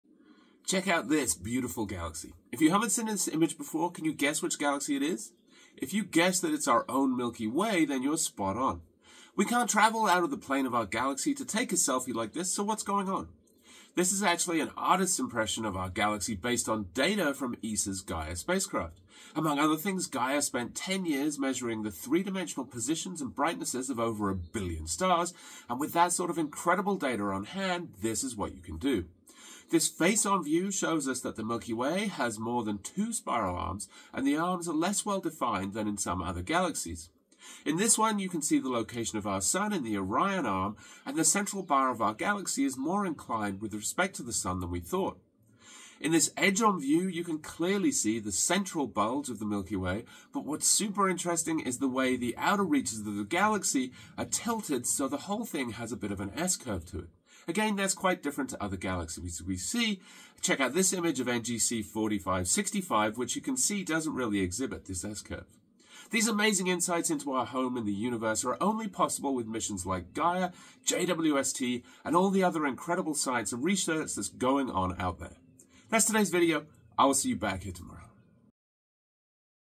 This Galaxy Looks Like a Cool Place to Live!
Check out what you can do with the amazing data from ESA's Gaia mission.